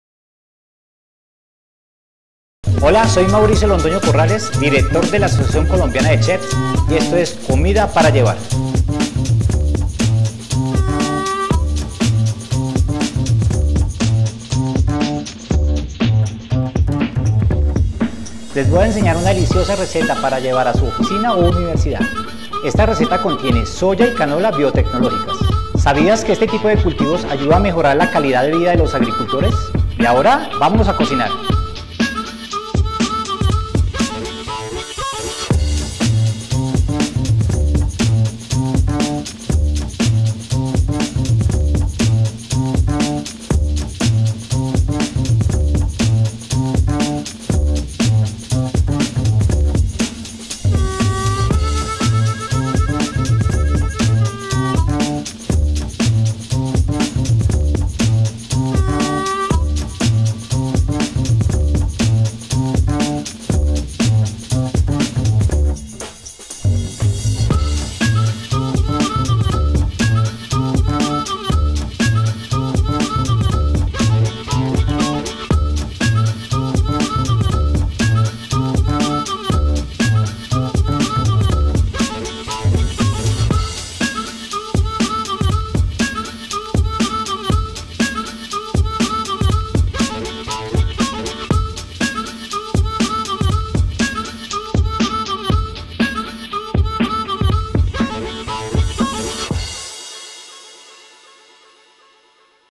Tortillas de chorizo de pollo
El chef Mauricio Londoño nos trae esta fácil, deliciosa y saludable receta para llevar a la oficina o universidad ¡Disfruta tu lonchera!